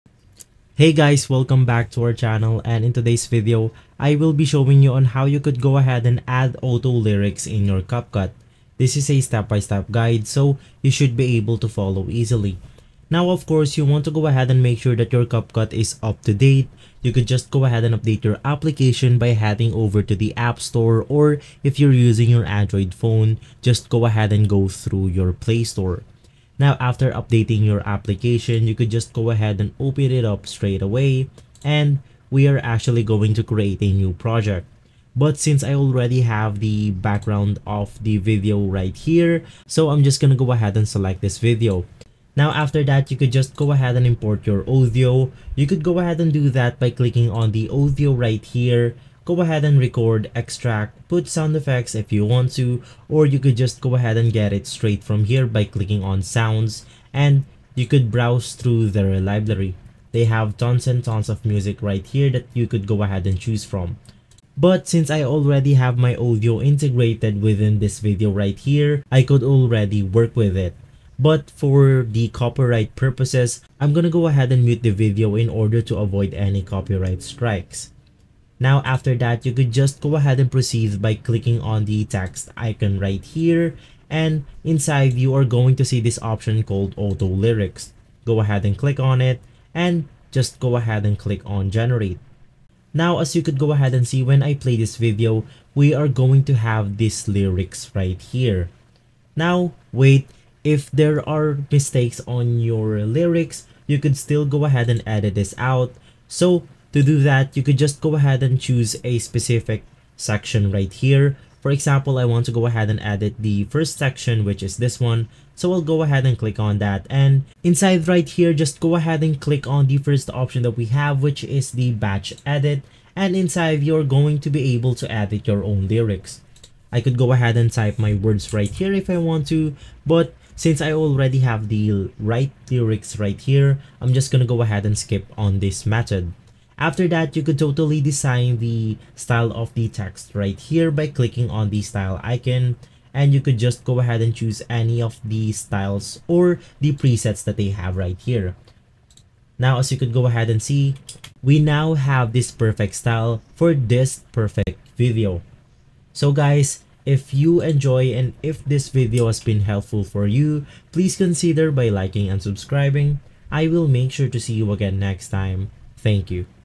How To Add Auto Lyrics In CapCut (Easy Tutorial)
Let's learn how to add lyrics automatically to your videos in CapCut. Wheter it's a TikTok video, Music Video, YouTube Shorts or whatever it is, you can add auto lyrics in seconds.